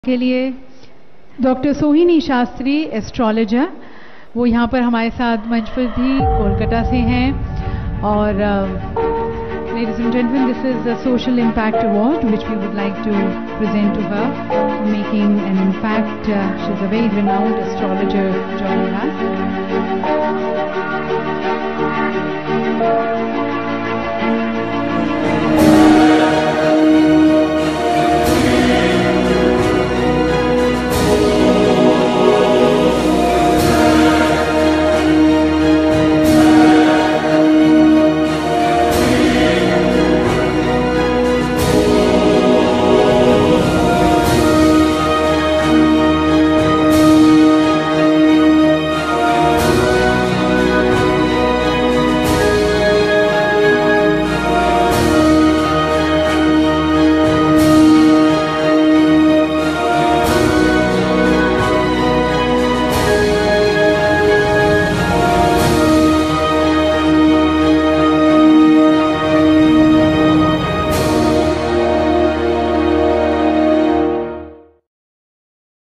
Pratigya - Stand for a Cause || SOCIAL IMPACT AWARD 2018 || DR. SOIHINI SASTRI || KIRRON KHER
Dr. Sohini Sastri, the best astrologer in India, received Social Impact Award 2018. Awarded by MP & Actress Kirron Kher for outstanding contribution to Indian Astrology.